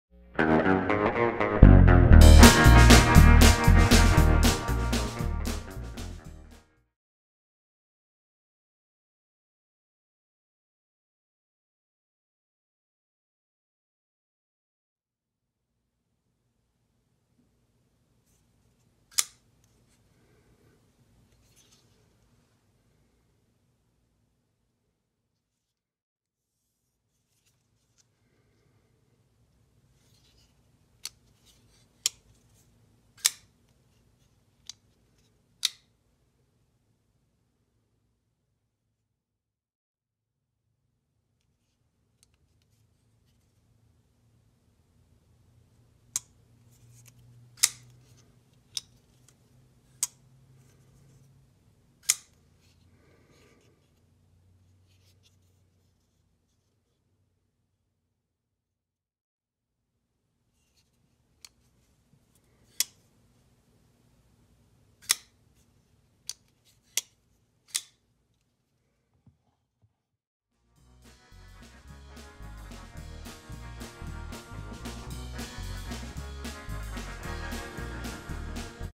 One Minute Knife Video: The CJRB Scoria
Here we have a recent addition to my rotation, the CJRB scoria. I think I like it even more than the Ria (my only other), what stands out to me is just how easy it is in the pocket and how thin over all.  I like.  You like? Sub!